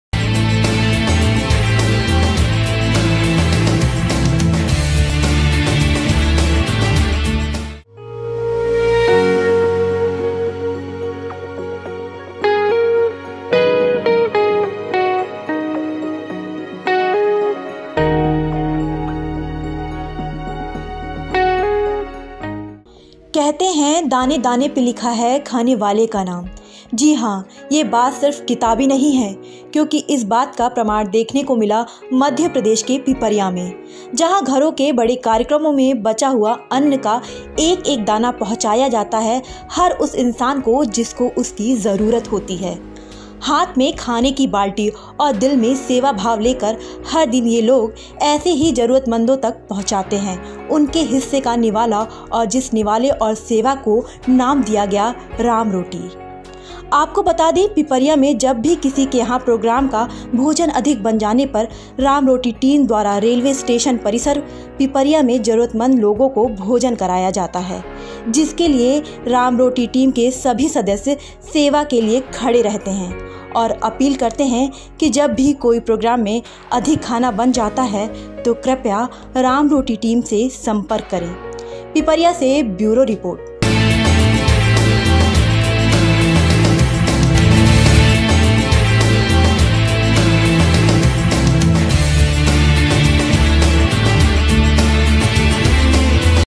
Hoshangabad राम की रोटी से भरता है पेट
कहते हैं दाने दाने पर लिखा है खाने वाले का नाम,,, जी हां ये बात सिर्फ किताबी नहीं है ,,,,क्योंकि इस बात प्रमाण देखने को मिला मप्र के पिपरिया में ,,जहां घरो और बड़े कार्यक्रमों में बचा हुआ अन्न का एक एक दाना पहुंचाया जाता है हर उस इंसान तक जिसको उसकी जरूरत होती है,,, हाथ में खाने की बाल्टी और दिल में सेवा भाव लेकर हर दिन ये लोग ऐसे ही हर जरूरतमंद तक पहुंचाते हैं उनके हिस्से का निवाला और जिस निवाले और सेवा को नाम दिया गया है राम रोटी,, आपको बता दें
पिपरिया जब भी किसी के यहां प्रोग्राम का भोजन अधिक बनजाने पर रामरोटीटीम द्वारा रेलवे स्टेशन परिसर पिपरिया में जरुरत मंद लोगों को भोजन कराया जाता है जिसके लिये राम रोटी टीम के सभी सदस्य सेवा के लिए खड़े रहते हैं और अपील करते हैं की जब भी कोई प्रोग्राम में खाना अधिक बन जाता है तो कृपया राम रोटी टीम से संपर्क करें पिपरिया से ब्यूरो रिपोर्ट